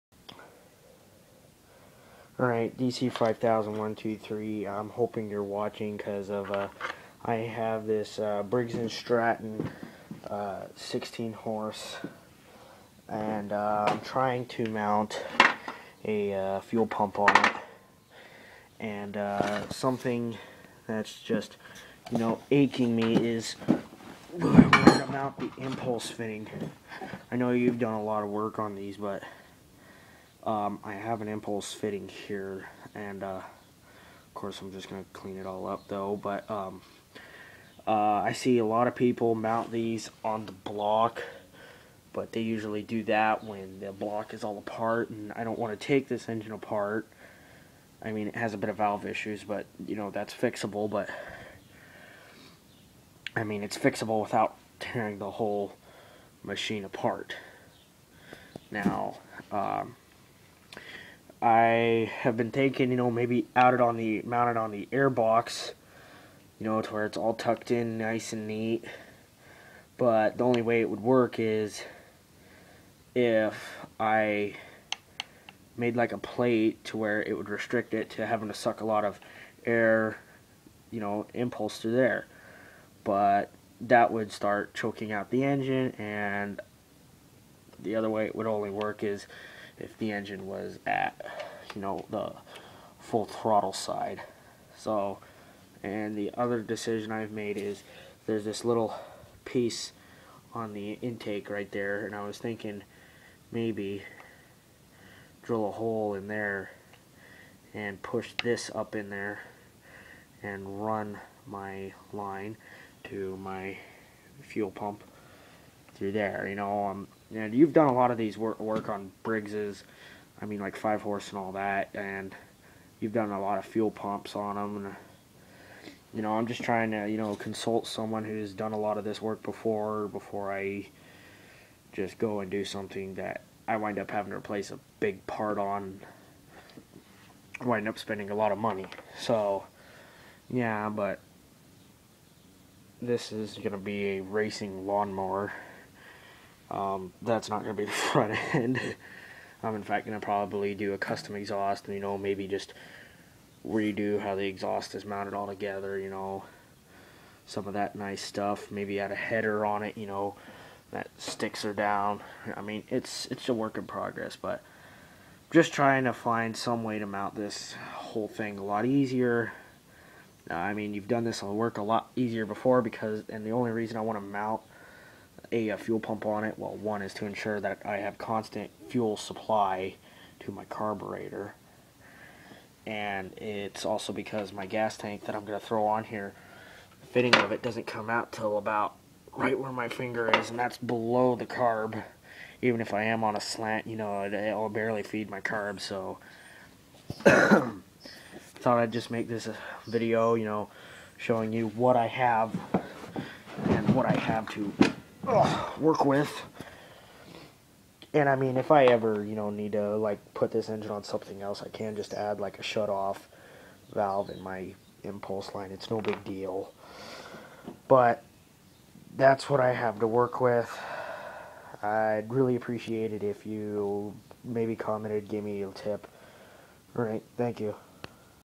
Fuel pump on Briggs 16hp bigblock
Need info on where to mount inpulse fitting on briggs